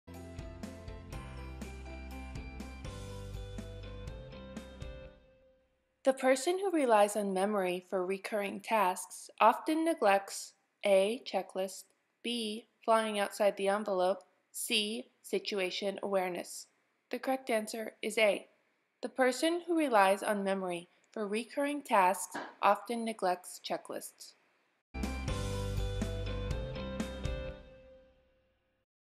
FAA Private Pilot Test Prep: The person who relies on memory for recurring tasks often neglects
The person who relies on memory for recurring tasks often neglects

a. checklists.
b. flying outside the envelope.
c. situation awareness.

 If you're interested in learning to fly, please consult with a CFI in your area.

Further Review: Aircraft Performance

Bookmark my private pilot playlist for more FAA knowledge test questions!

Thank you for your likes, comments, and subscriptions!